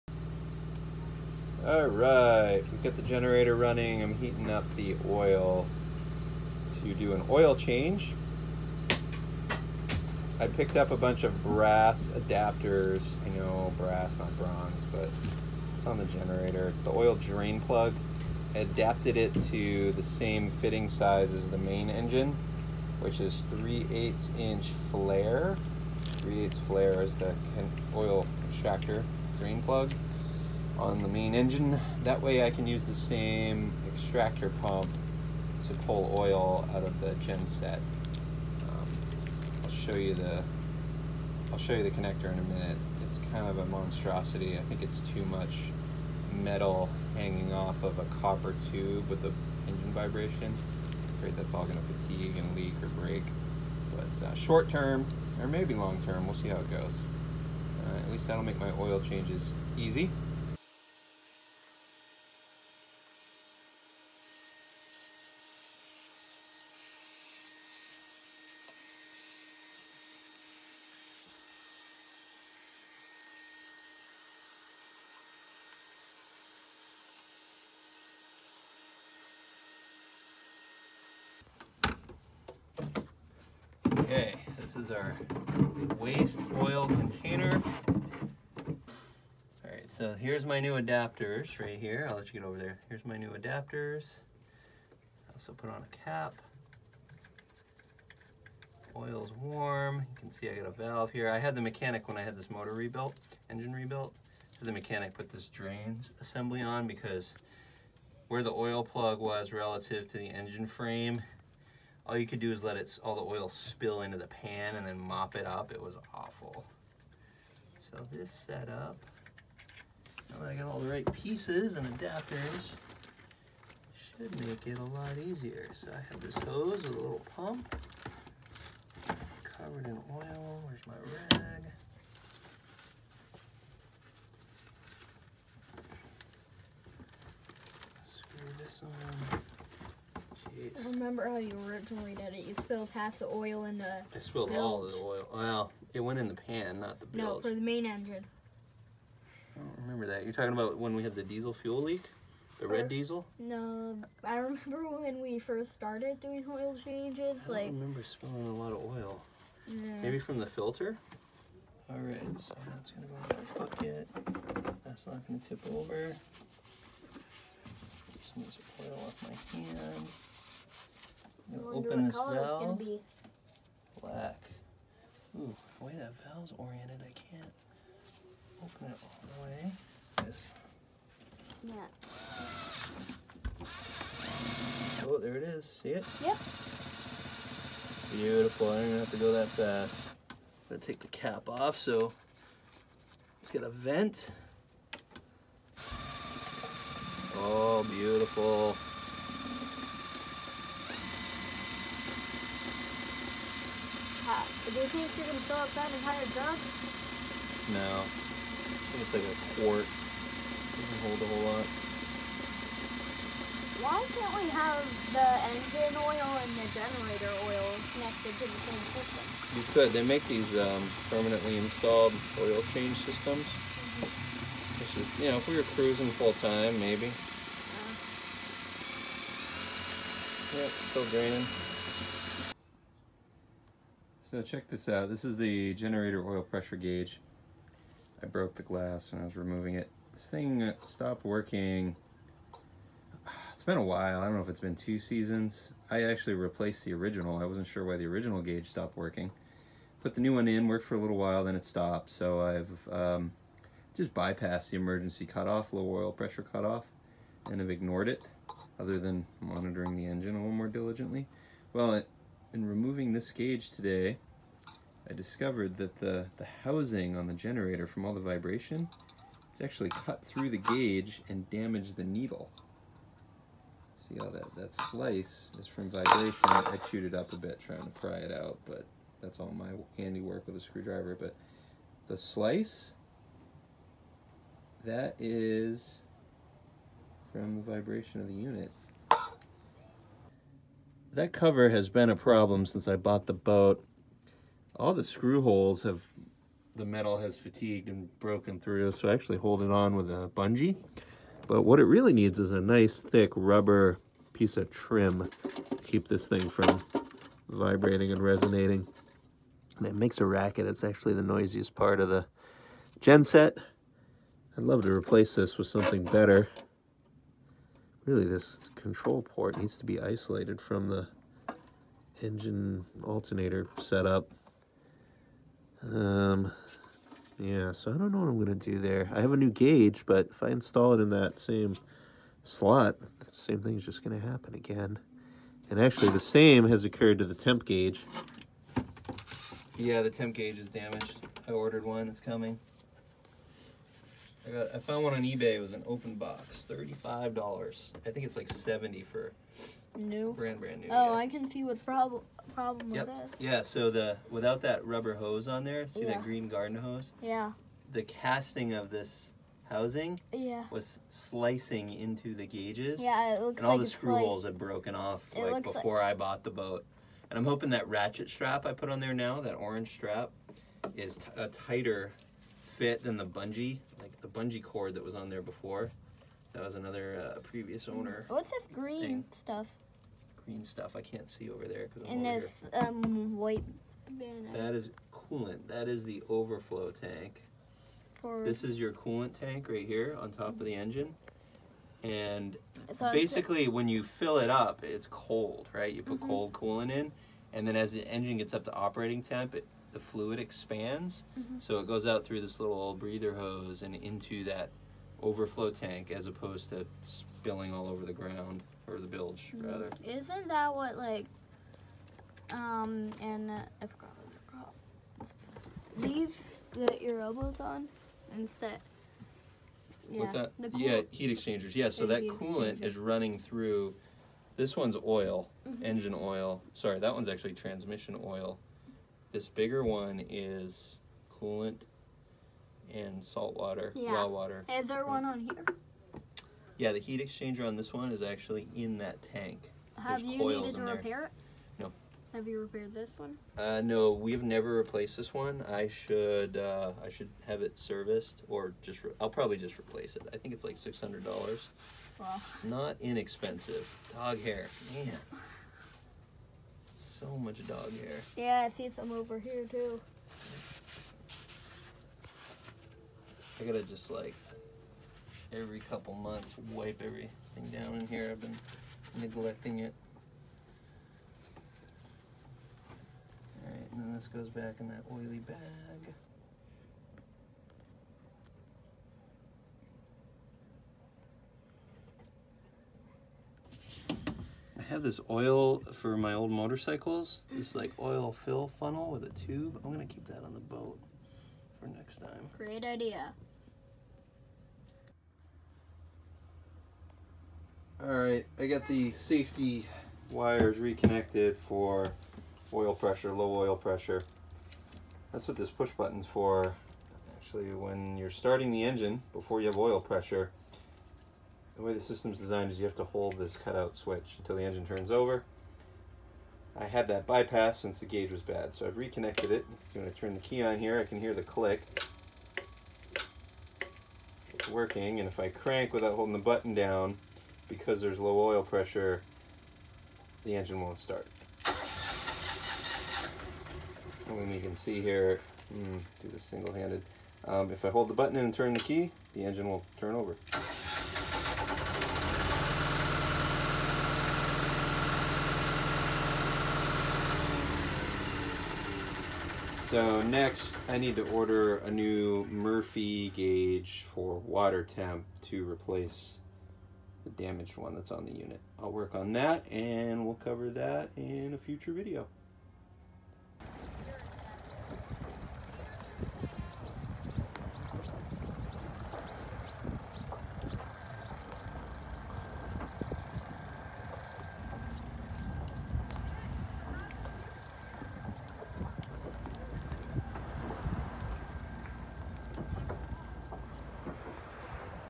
Generator damage and oil change mods
Maintenance to a Kubota 300 series marine diesel generator.